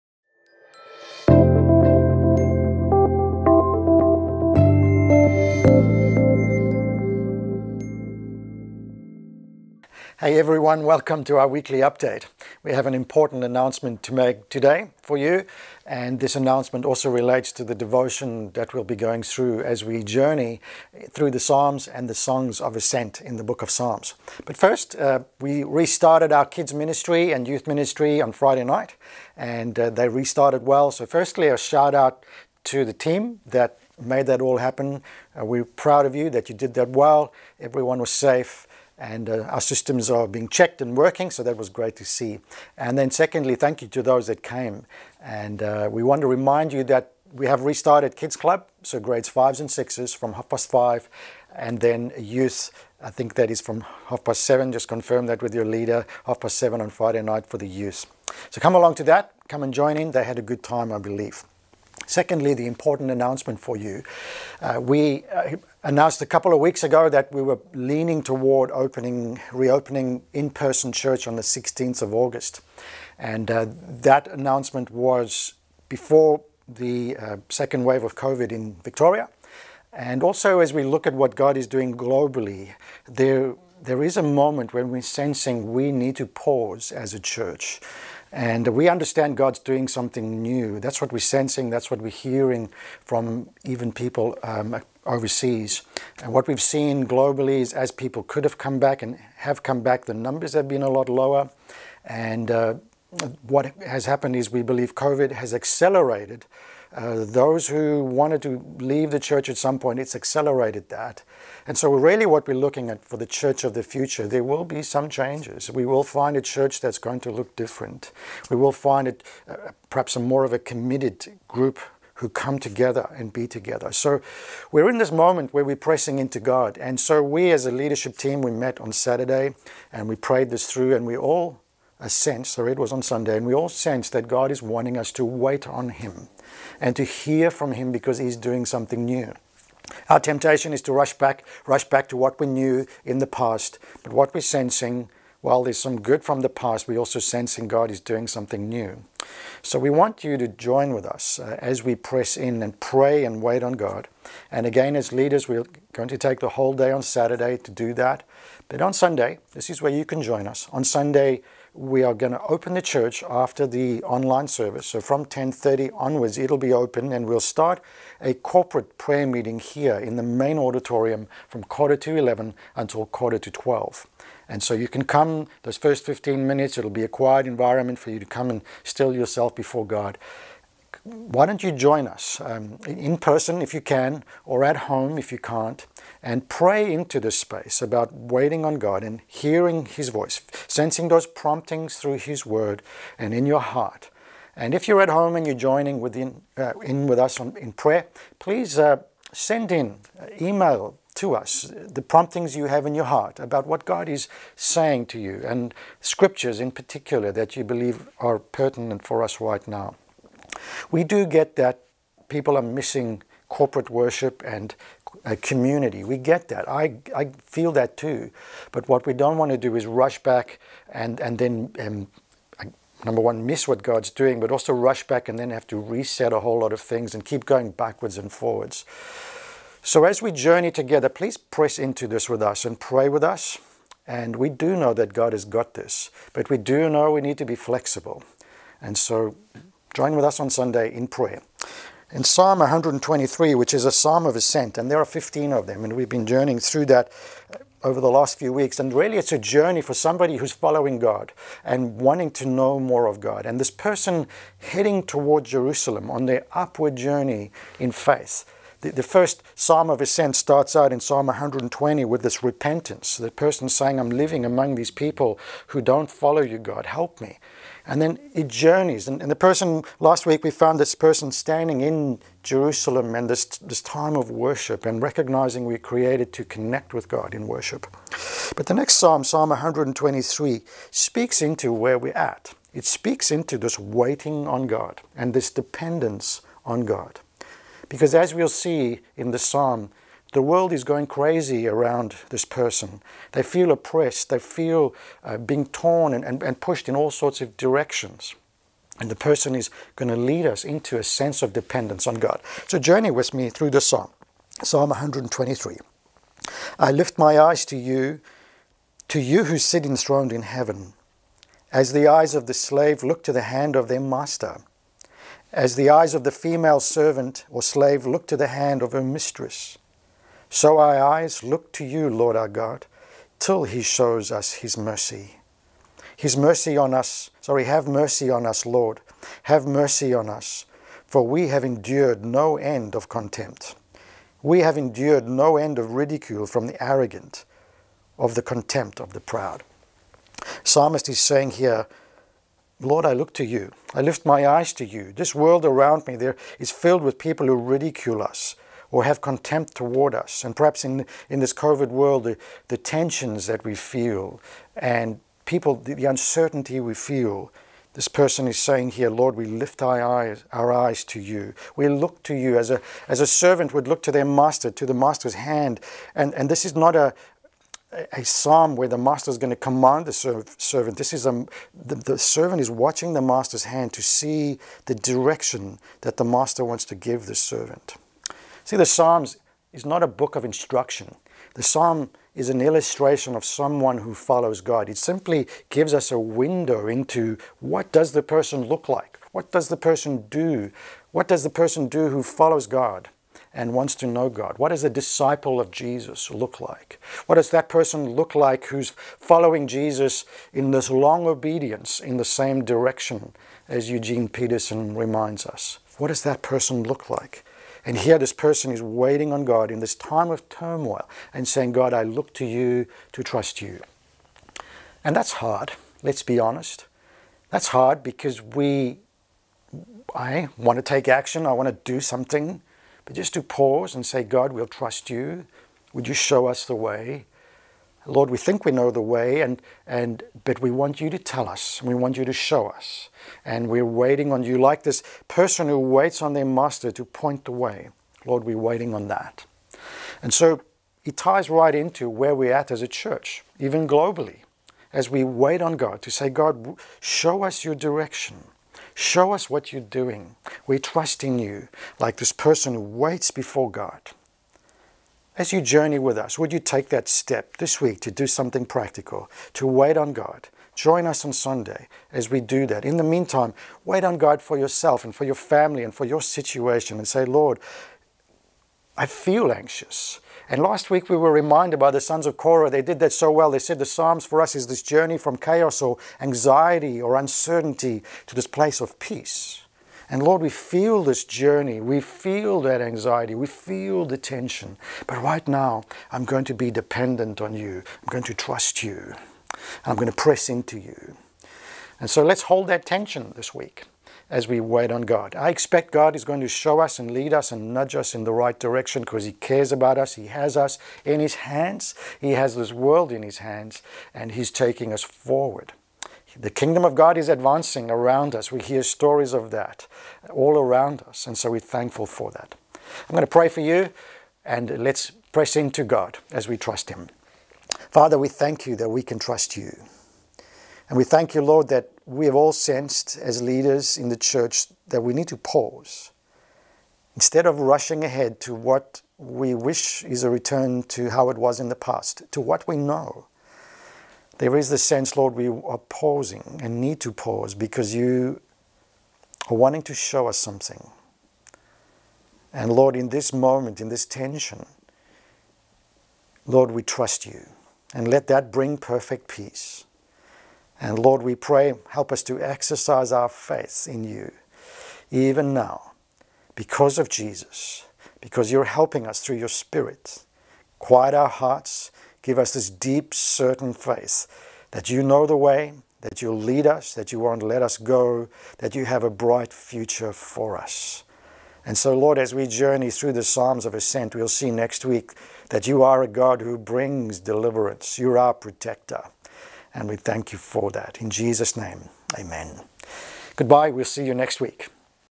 Songs of ascent | Weekly Update | 29 July 2020
KIDS & YOUTH
Find out all you need to know about Kids Club* and Youth starting on Friday.
*At this stage, Kids Club is open to Grades 4 & 6 only.

HERVEY BAY BAPTIST CHURCH | Live on Sundays at 9:30am

ANY QUESTIONS?